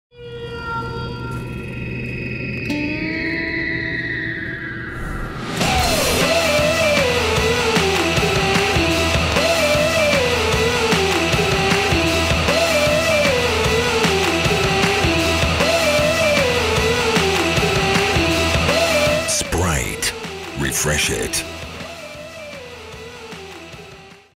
ofelia מתרעננת בהילוך איטי בפעילות של ספרייט
ספרייט יצאה לסיבוב בחופי הארץ כדי לצלם אתכם מתרעננים בהילוך איטי מבלון מים לפנים!
רוצים סרטון משלכם? חפשו "ספרייט" בפייסבוק או לחצו כאן